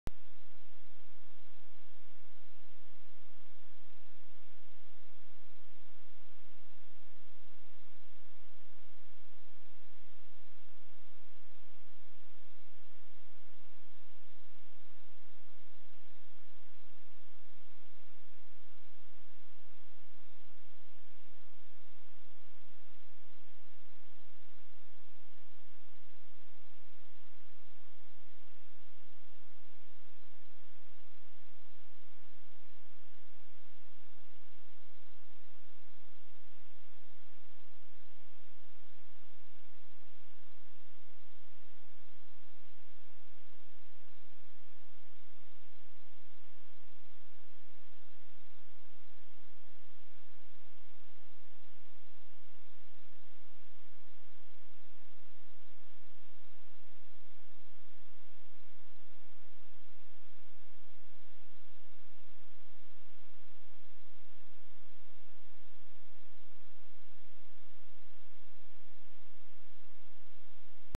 TCR 260 rattitude flight mode test
so much interference here.every time i turn on video is different.